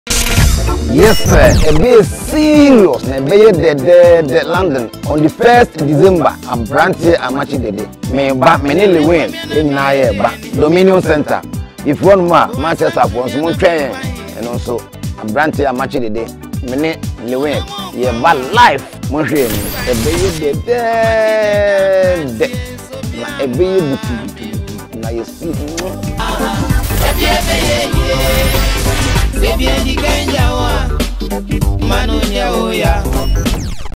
Amakye Dede : Live in London & Manchester
Salt Media Gh & Mayday Entertainment presents

                     LONDON
Date: Saturday 1st December 2018
Venue: Dominion Centre,
London N22 6DS
Time: 7PM - Late
                 MANCHESTER
Date: Friday, 7th December 2018
Venue: Sol House
Time: 8PM - Late
Official Host - @countrymansongo
Performing Live:

More to be confirmed.

Dj’s on the night: @dejaytcue
Supporting Mc: DA FOSS, NKOSUOHENE & OBUDO

ARE U READY FOR A AMAZING NIGHT ! @amakyedede 🔥🔥🔥